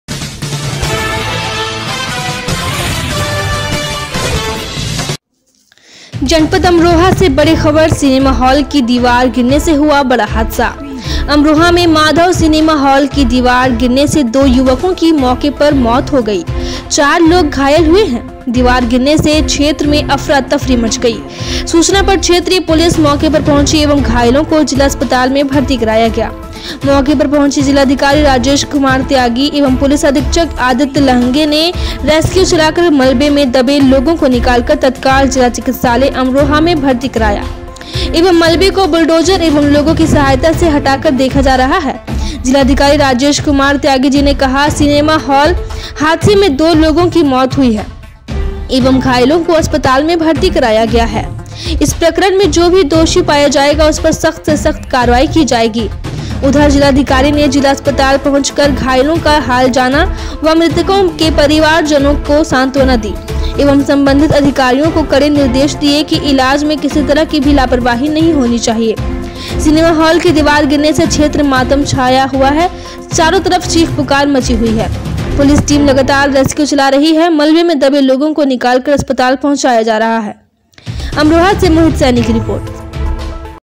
अमरोहा से बड़ी खबर सिनेमा हॉल की दीवार गिरने से हुआ बड़ा हादसा
अमरोहा में माधव सिनेमा हॉल की दीवार गिरने से दो युवकों की मौके पर मौत हो गई 4 लोग  घायल